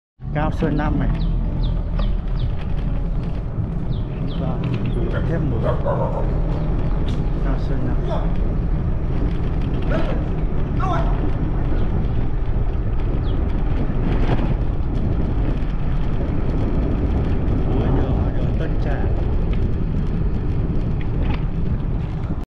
Đường Cao Sơn 5 Phường Hòa Hải Quận Ngũ Hành Sơn Thành Phố Đà Nẵng
Khám phá Đường Cao Sơn 5 Phường Hòa Hải Quận Ngũ Hành Sơn Thành Phố Đà Nẵng trong video này. Được biết đến với vẻ đẹp tự nhiên và yên bình, Đường Cao Sơn sẽ mang đến cho bạn những trải nghiệm thú vị khi đến Đà Nẵng!